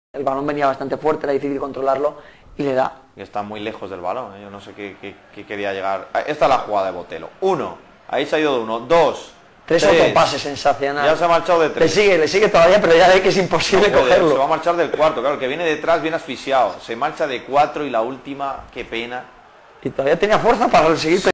Jugadón de Pedro Botelho en el Helmántico
HalaUnion.com

Pedro Botelho recorre el césped de área a área desbordando a 4 contrarios y plantándose sólo delante del portero Tinerfeño en la victoria por 1-0 de la Unión Deportiva Salamanca frente al Club Deportivo Tenerife.